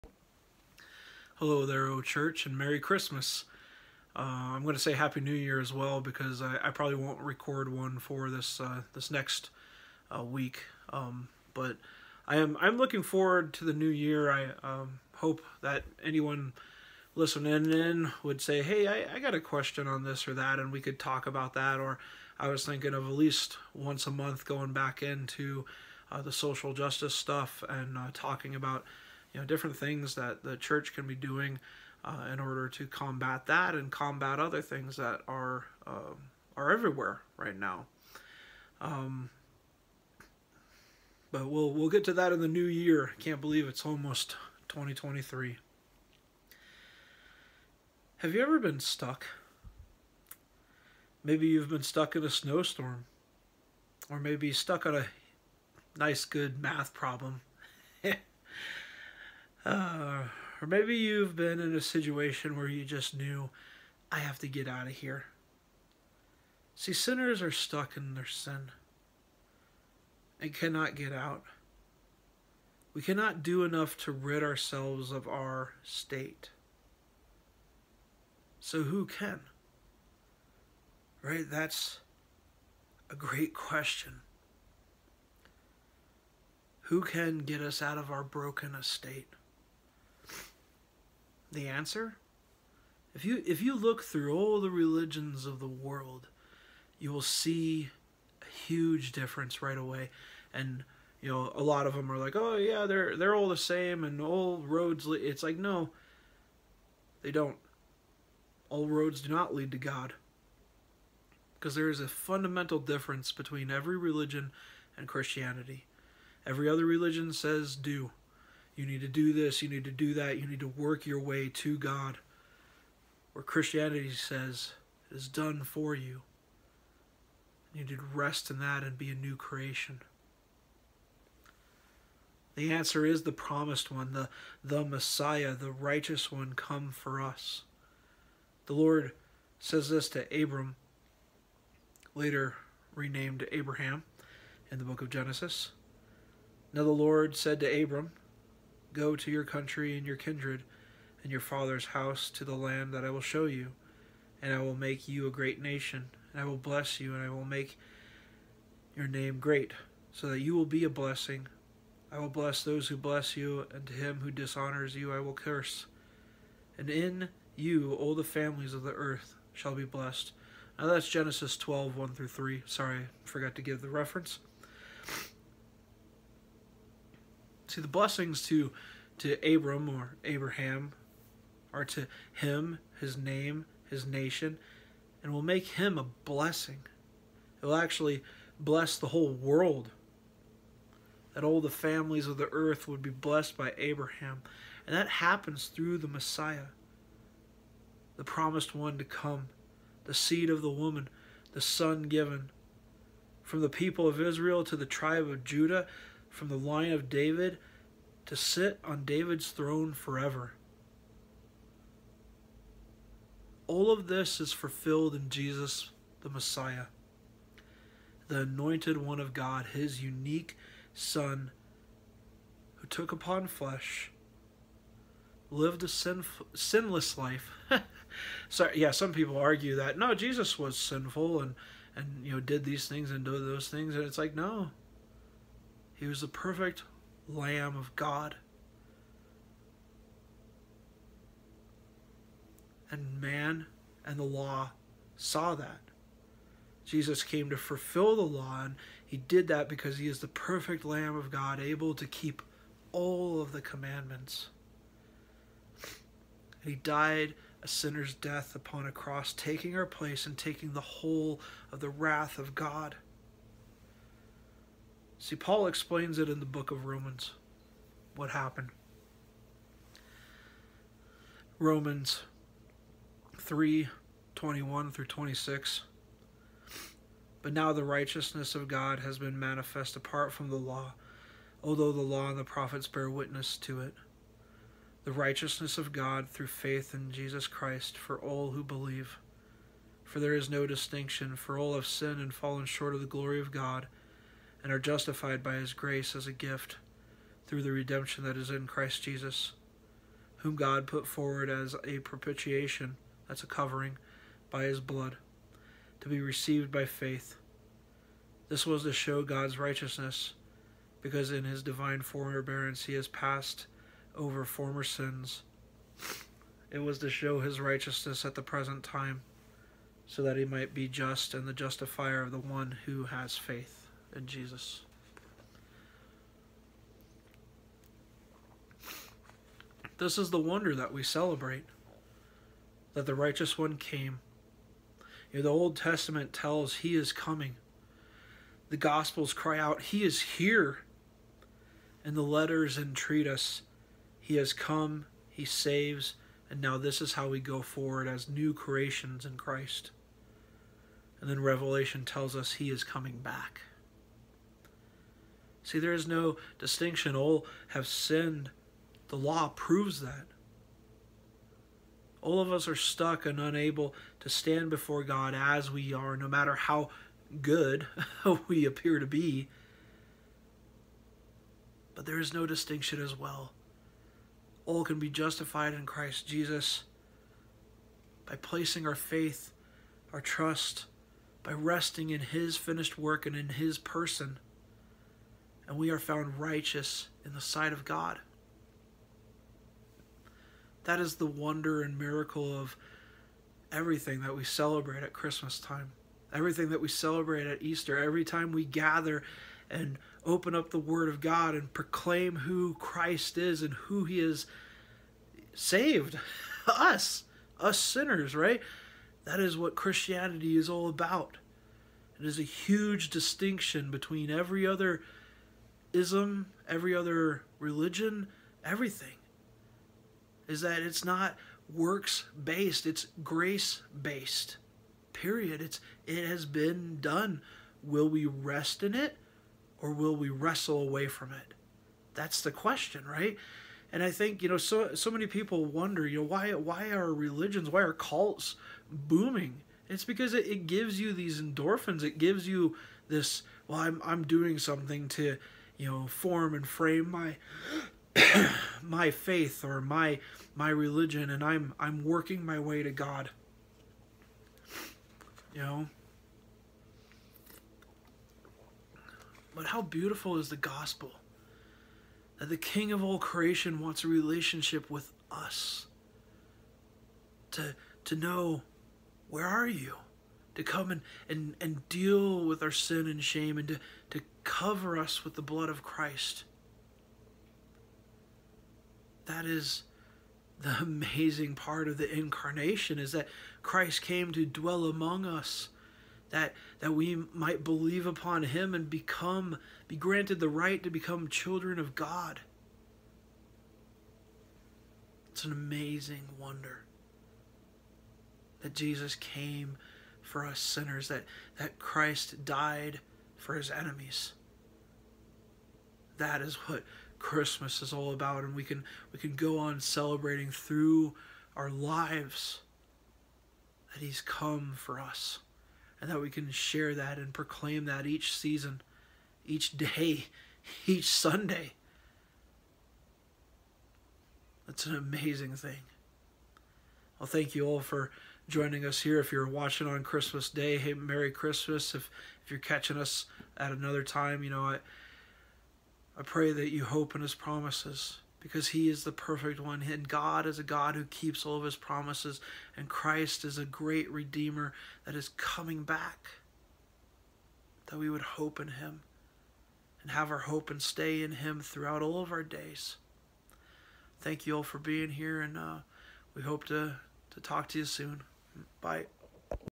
The promised Hope
Join us this Christmas season as we celebrate the birth of Christ Jesus and study scripture. We will look at Genesis 12:1-3 and Romans 3:21-26 today.